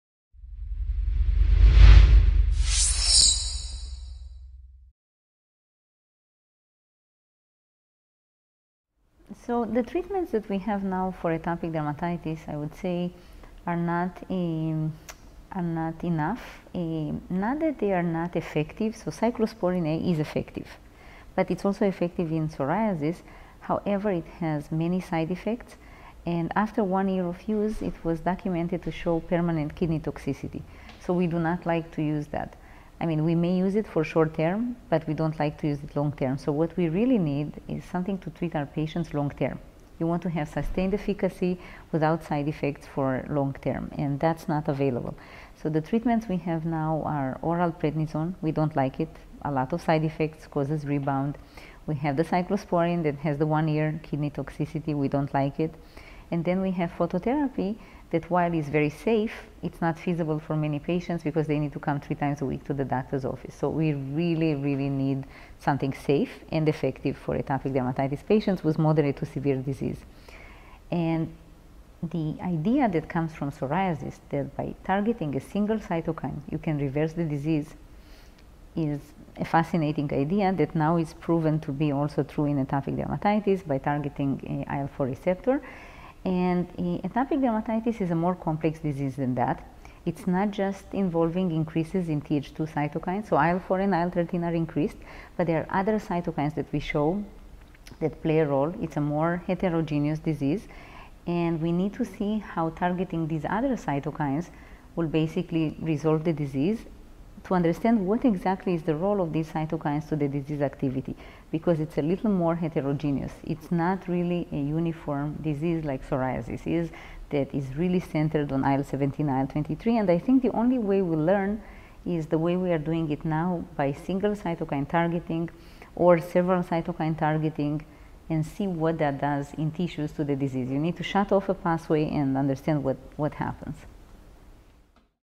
Dr Emma Guttman-Yassky on Current and Future Atopic Dermatitis Treatments
In the search for a safe and effective treatment for atopic dermatitis (AD), new research looks at the immune pathway of psoriasis to potentially help target AD, according to Emma Guttman-Yassky, MD, PhD, of the Icahn School of Medicine at Mount Sinai Medical Center in New York.